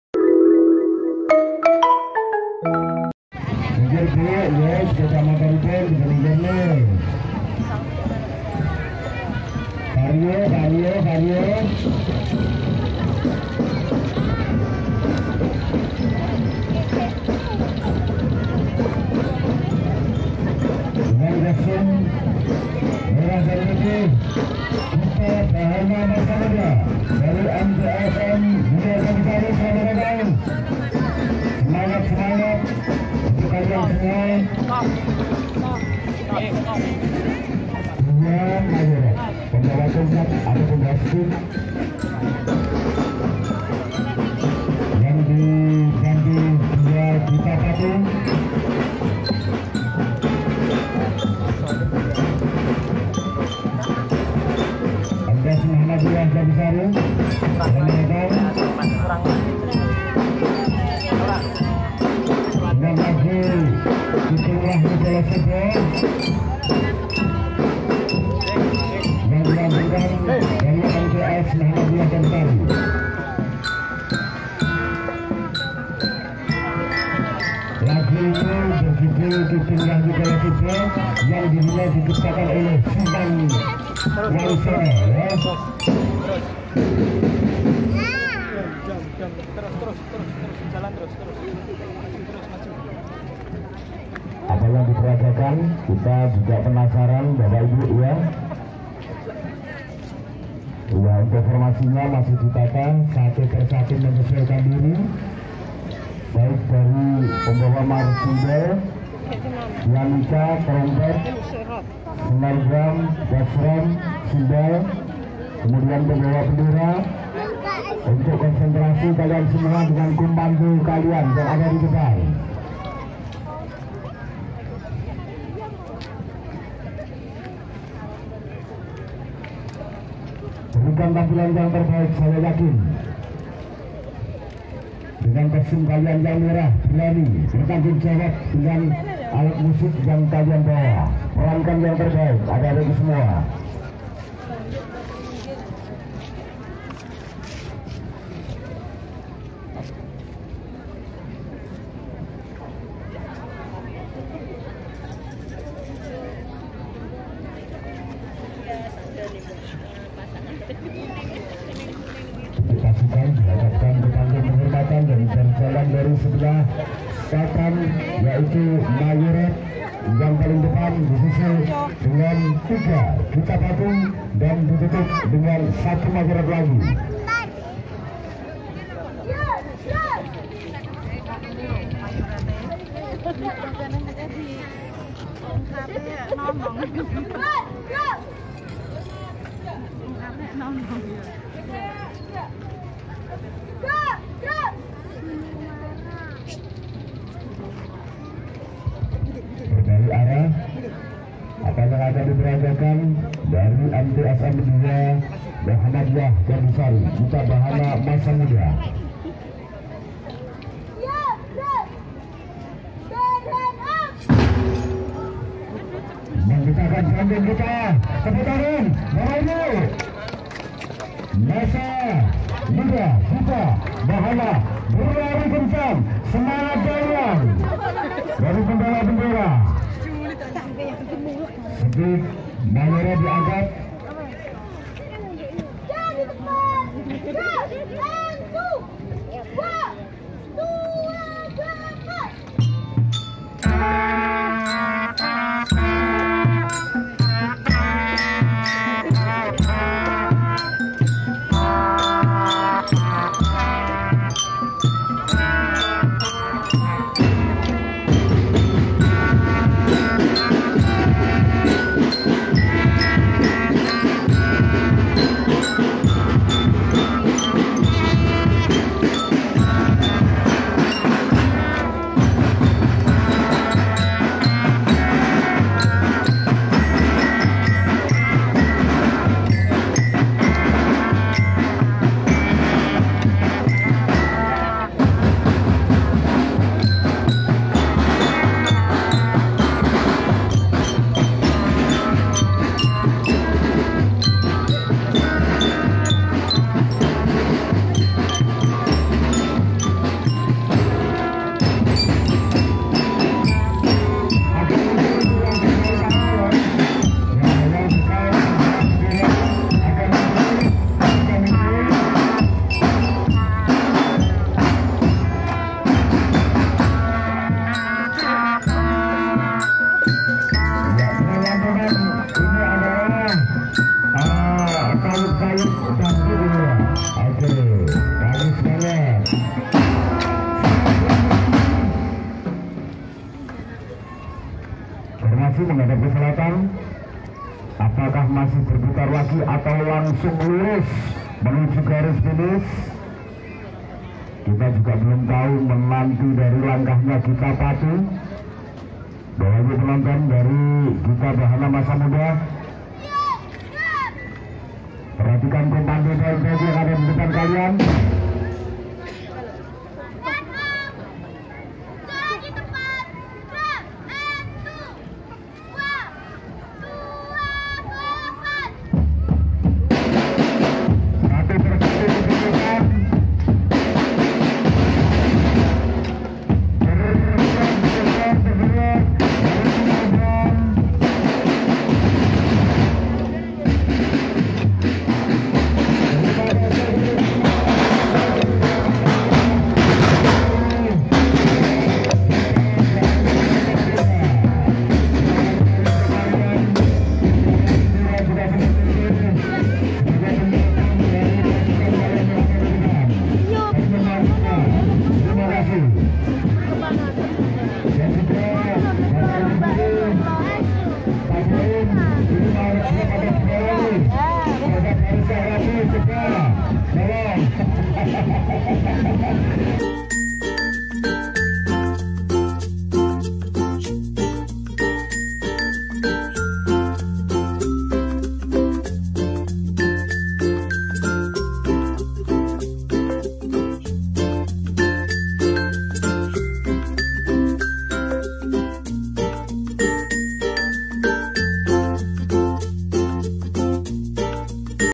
ATRAKSI PARADE DRUMBAND MTS MUHAMMADIYAH 2 GANDUSARI TRENGGALEK
Parade drumband kecamatan gandusari trenggalek pada hari kamis,22 agustus 2019